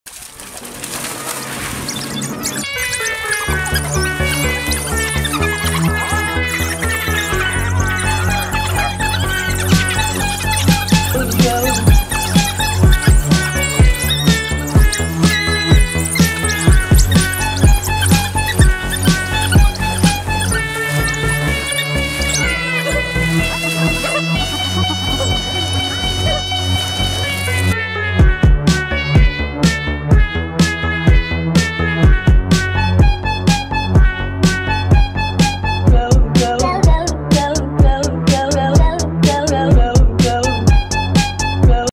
Best epic underwater battle. Impala alone fights with a pack of vicious wild dogs.
Best epic underwater battle. Impala alone fights with a pack of vicious wild dogs. Wild Animals Attck.